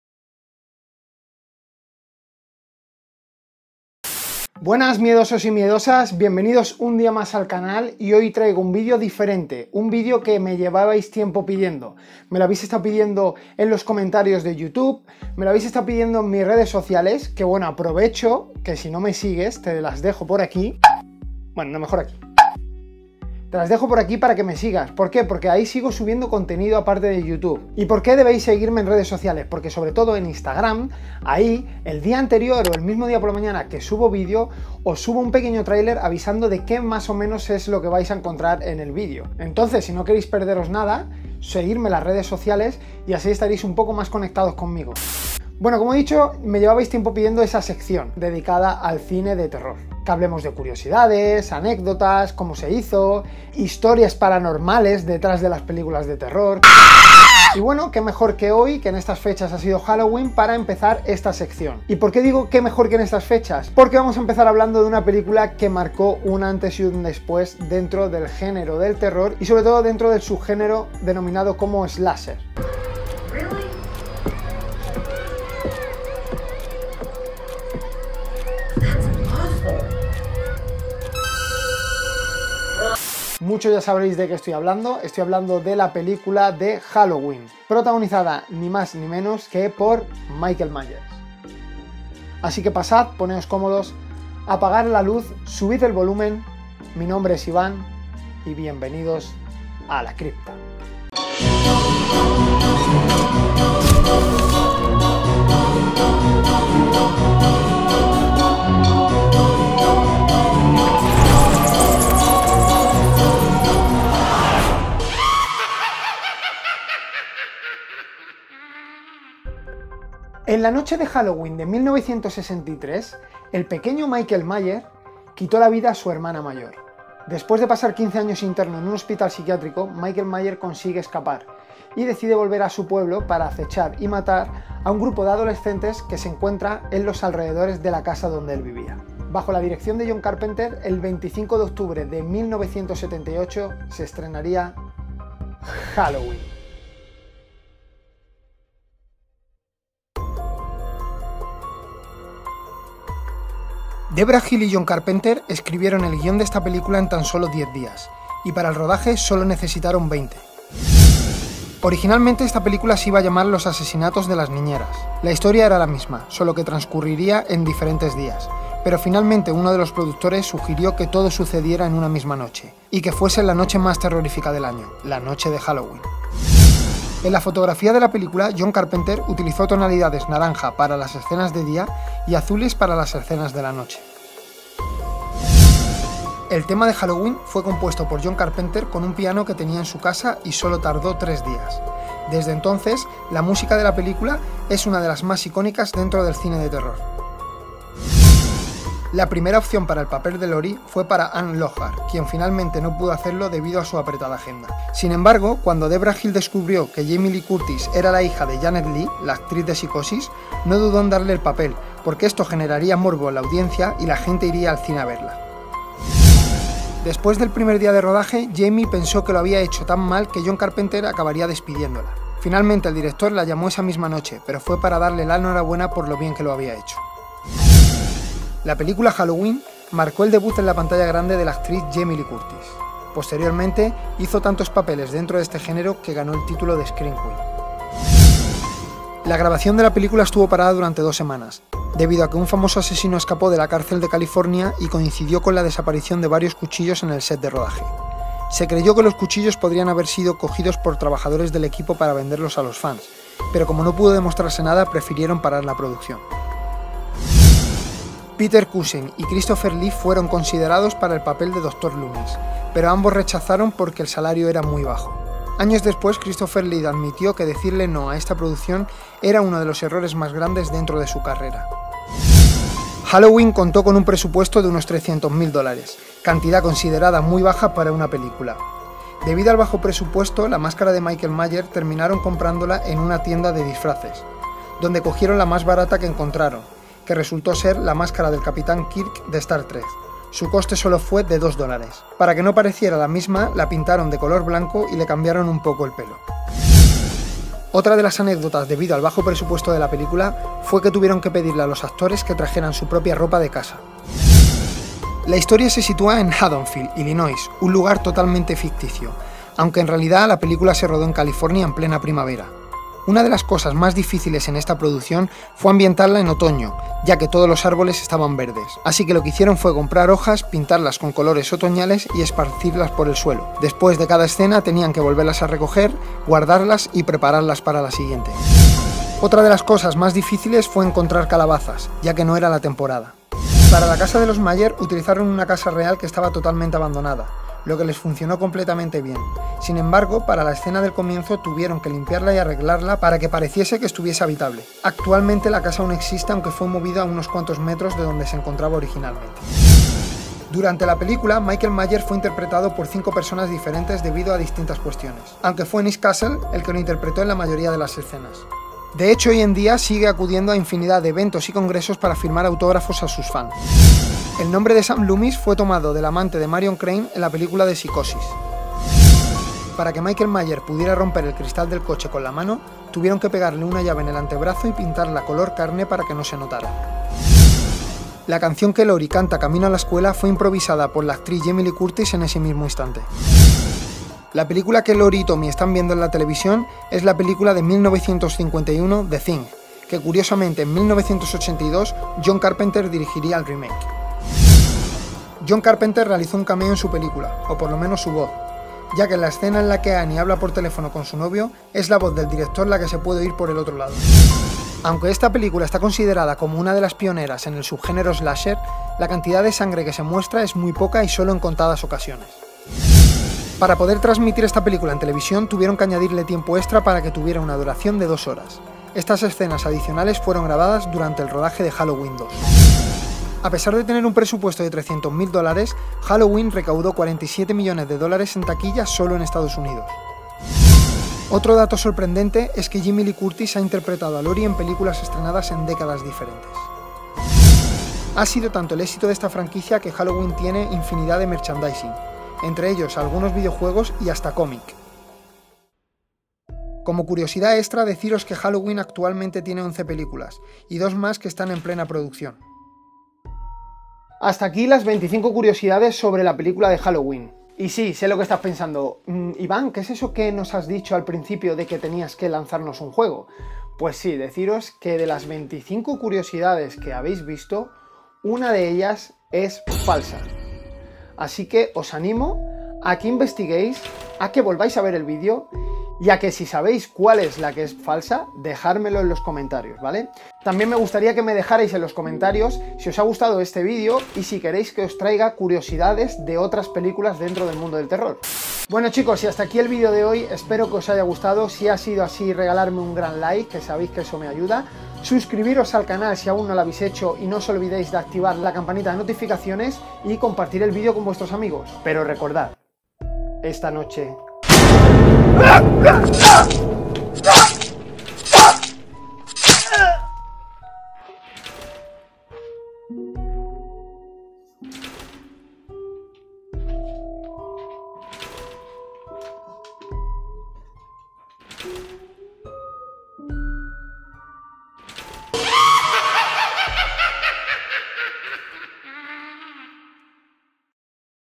25 CURIOSIDADES de🔪HALLOWEEN🎃 que quizás NO SABÍAS (Cine de Terror)
25 CURIOSIDADES de🔪HALLOWEEN🎃 que quizás NO SABÍAS (Cine de Terror)

Adelante! Apaga la luces, sube el volumen y bienvenidos a La Cripta.